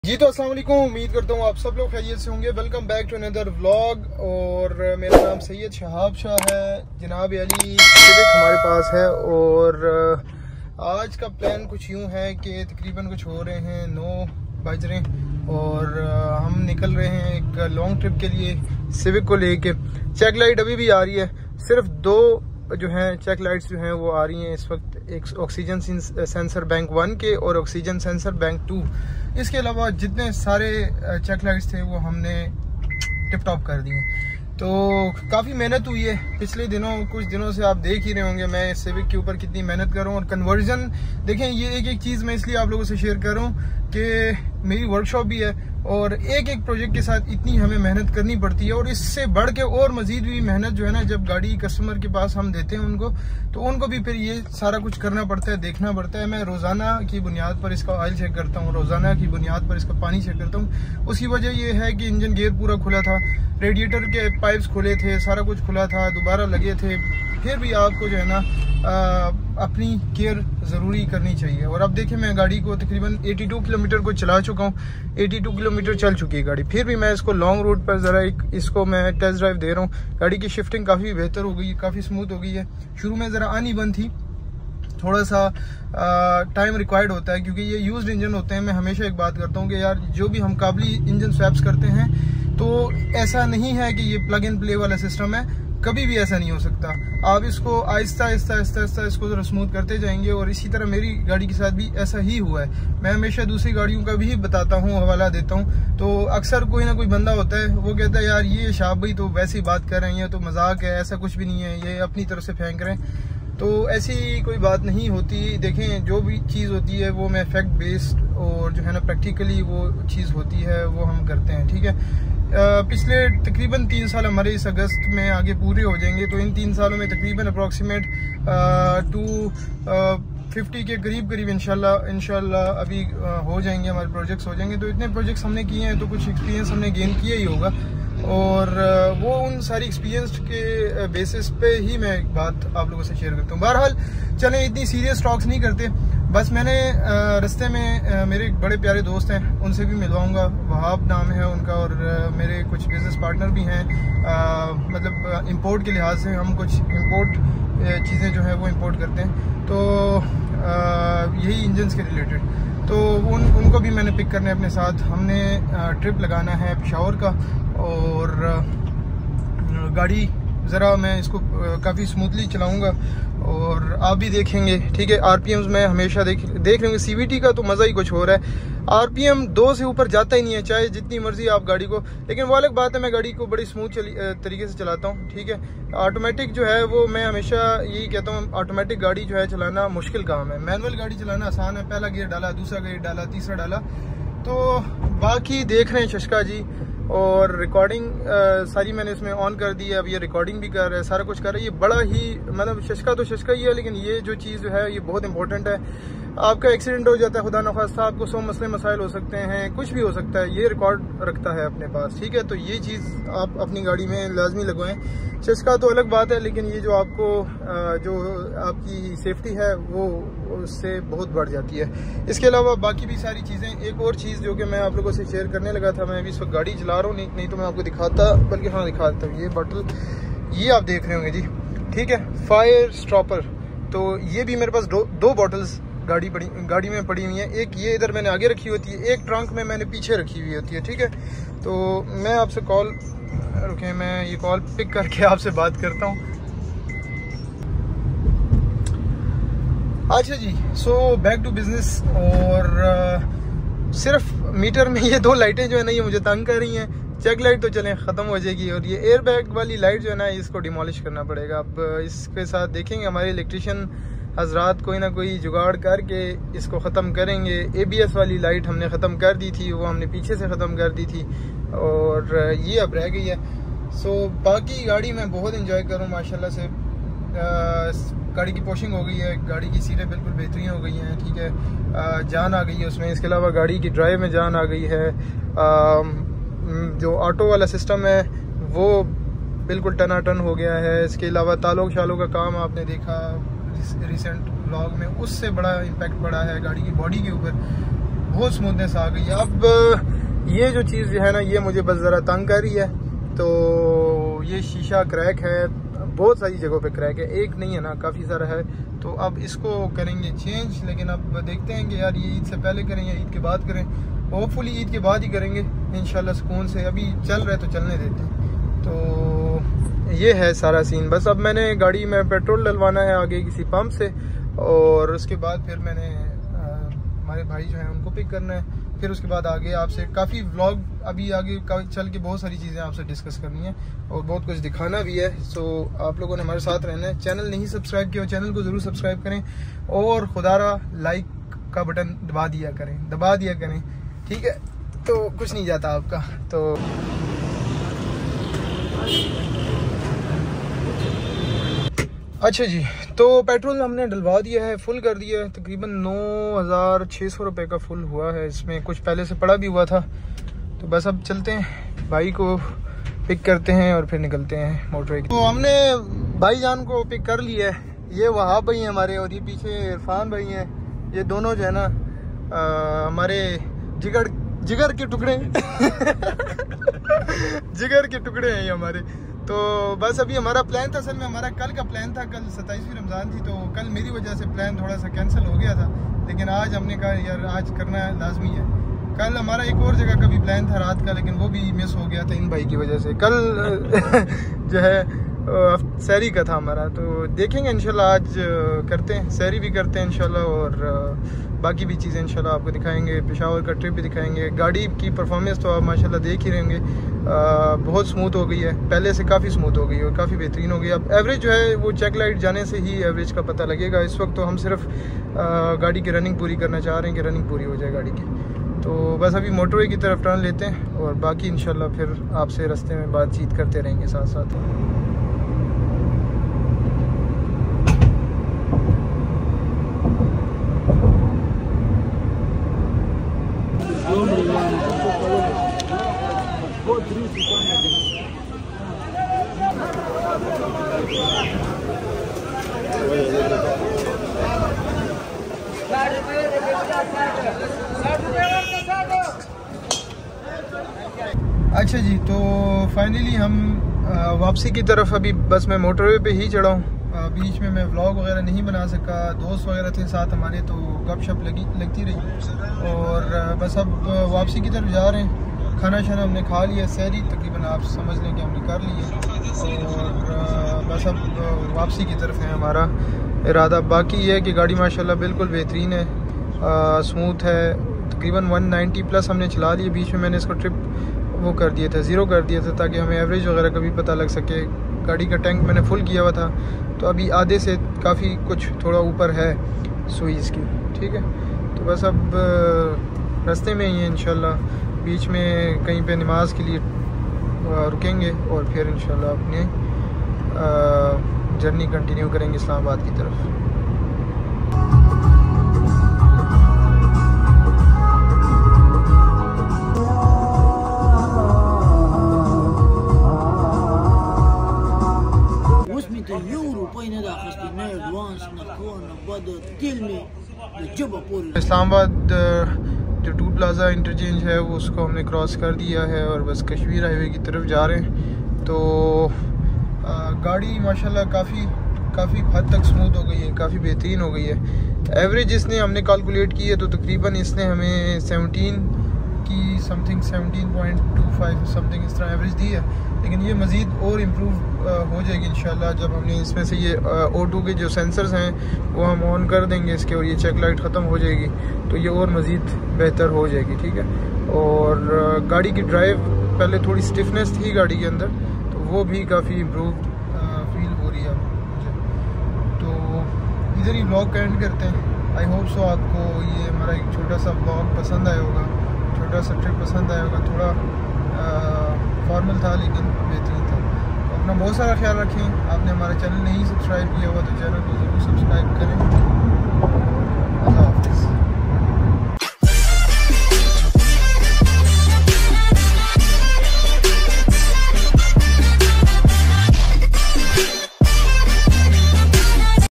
Auto Civic First Long Run 💯 | Honda Civic Manual to Automatic Conversion part 7
Honda civic manual to automatic conversion